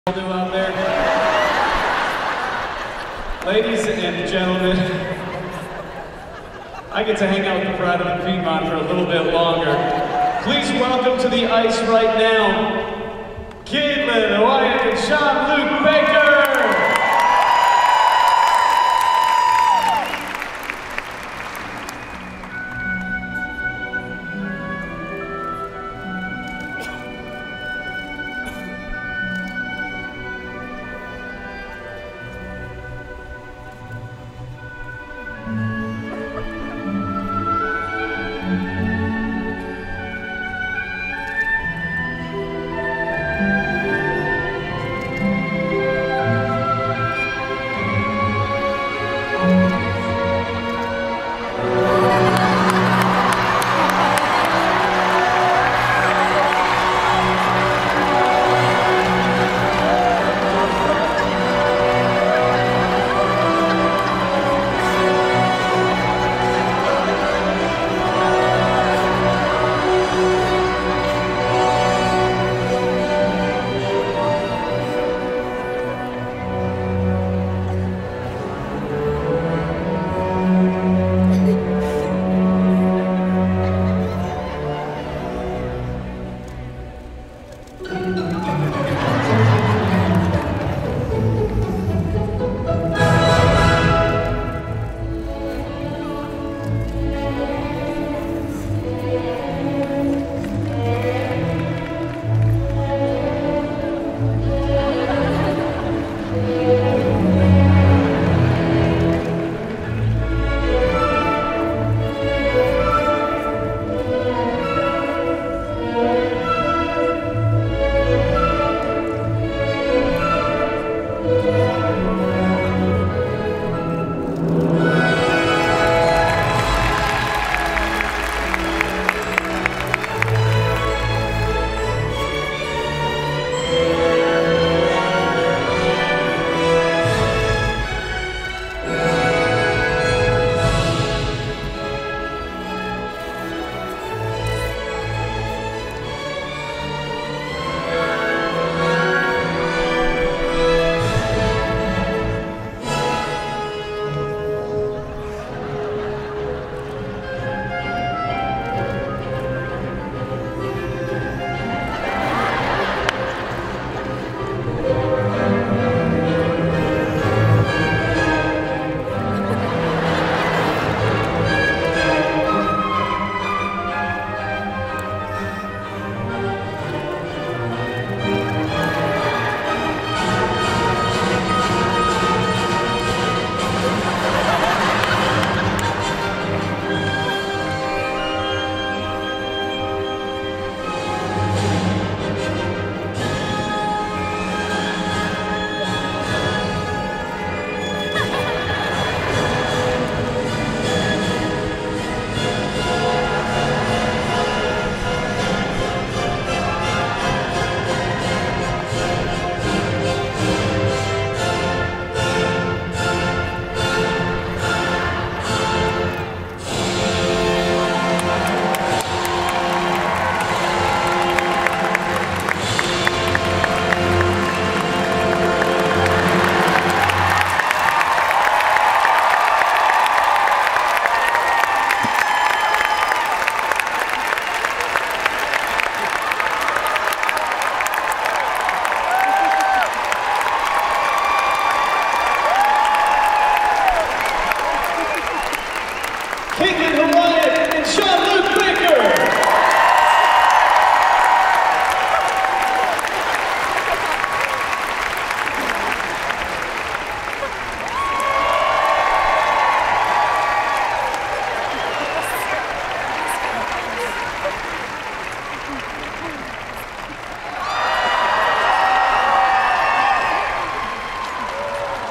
Kaitlin Hawayek and Jean-Luc Baker in Skating Spectacular Show for US Figure Skating Championship
The funniest show of the night!